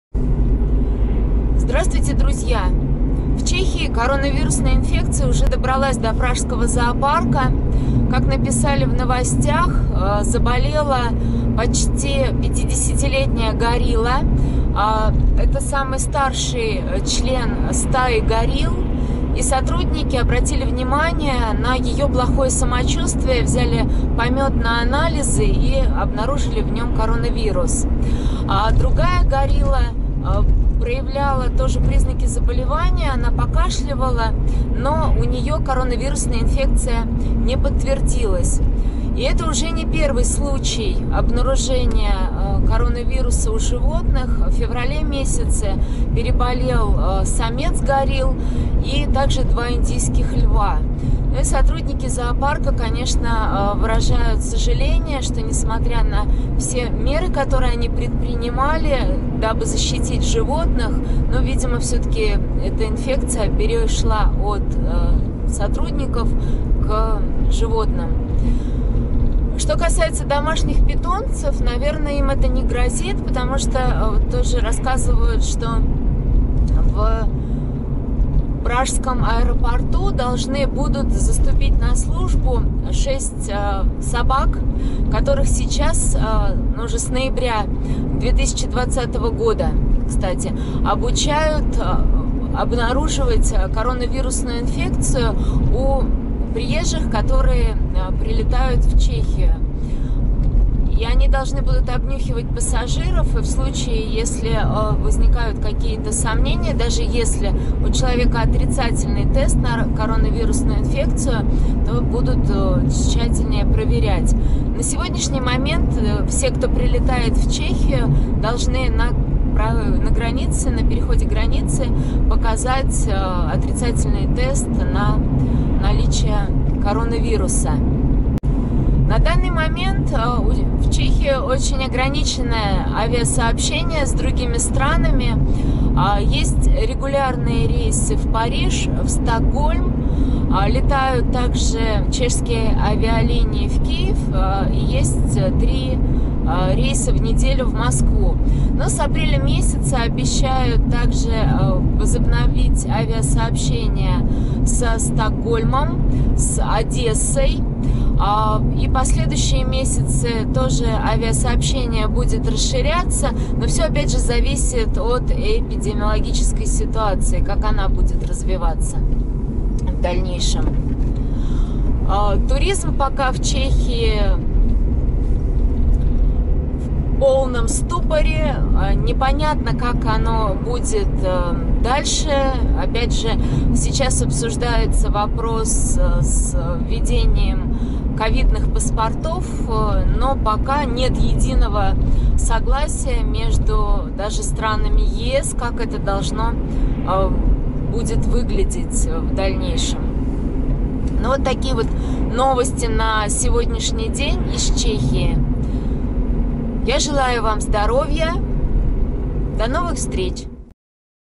Коронавирус Чехия. Новости Чехии 23.03.21 Коронавирус добрался до Пражского зоопарка.
Здравствуйте, друзья! Рада приветствовать вас на моем канале
На моем канале вы узнаете о жизни в Чехии и Европе.
Сovid-19 затронул и Пражский зоопарк

Тестирование животных, осуществляемое по образцам кала, сегодня выявило коронавирус у азиатских львов Джамвана и Сучи и у гориллы-самца Рихарда. Их симптомы пока что очень лёгкие,...